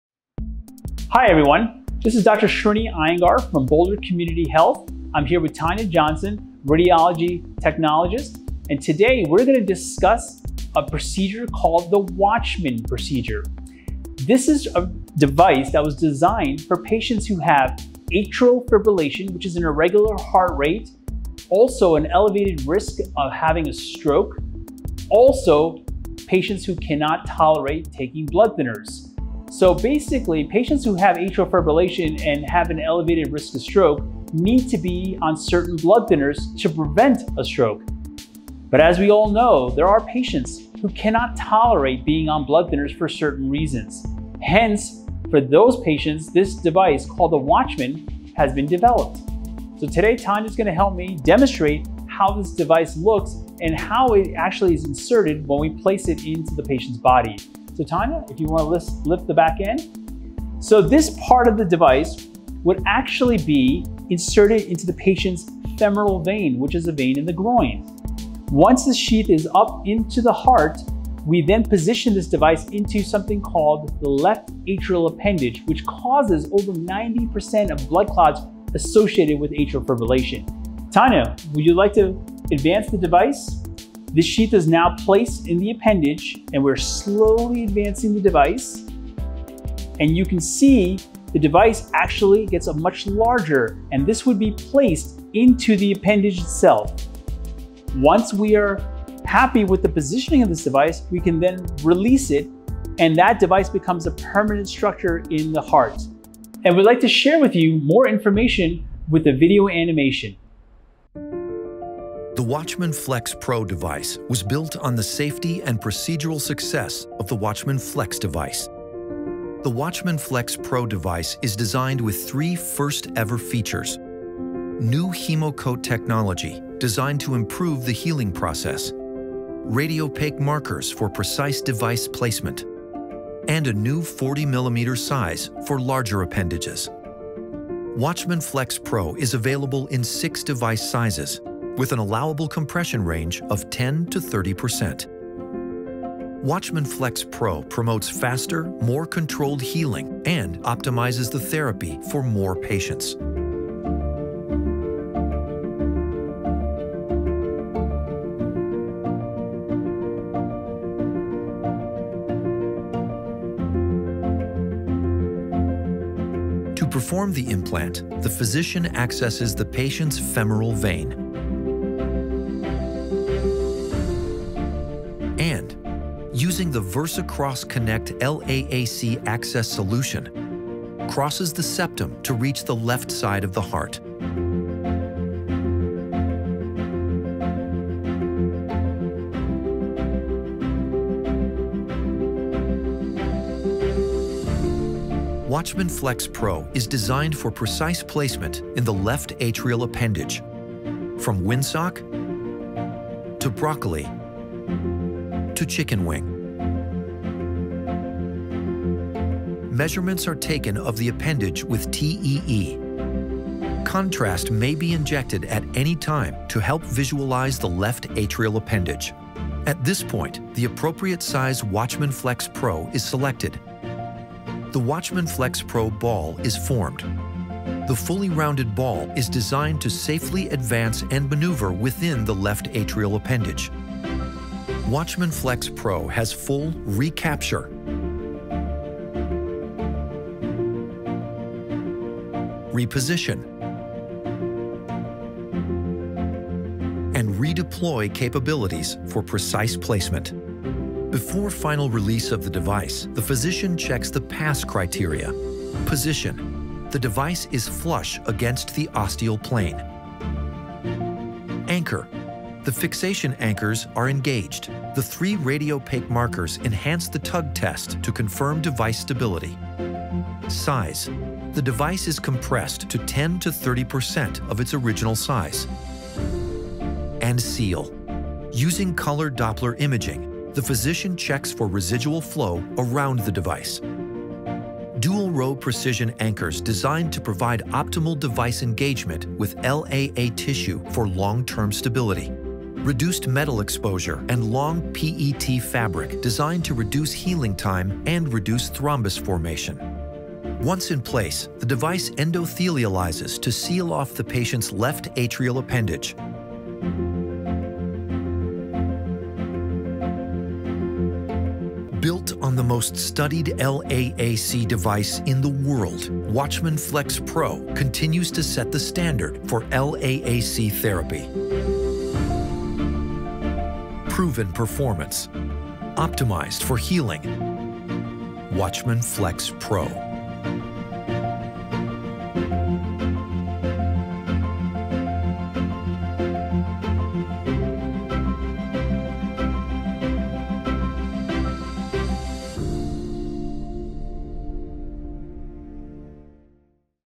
How the WATCHMAN FLX™ Pro procedure treats AFib and reduces risk of stroke | BCH Boulder Heart
The WATCHMAN™ device is the only FDA-approved implant proven to reduce stroke risk in people with atrial fibrillation (AFIB) that is not caused by a heart valve problem. Foothills Hospital was the first hospital in Colorado (full market release) to offer patients the newest technology - WATCHMAN FLX™ Pro. BCH’s WATCHMAN program is led by Srinivas Iyengar, MD – Director of Structural Heart & Interventional Cardiology. In this video, Dr. Iyengar demonstrates how he closes the left atrial appendage (LAA) by placing the WATCHMAN device inside the heart through a one-time minimally invasive procedure.